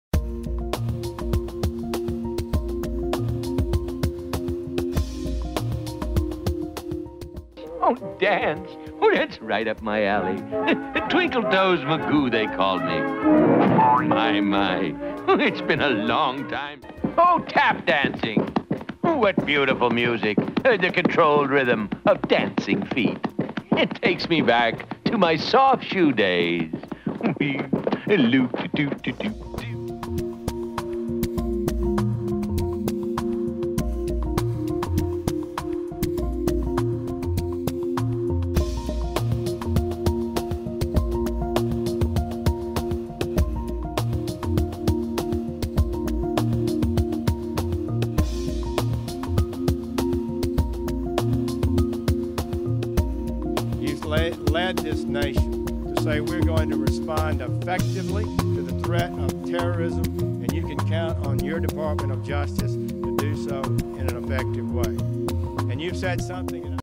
Trump refers to Sessions as 'Mr. Magoo.' Who is the character?
The Washington Post reported that President Trump refers to Attorney General Jeff Sessions as “Mr. Magoo” behind the scenes. Here’s some stuff to know about the cartoon character.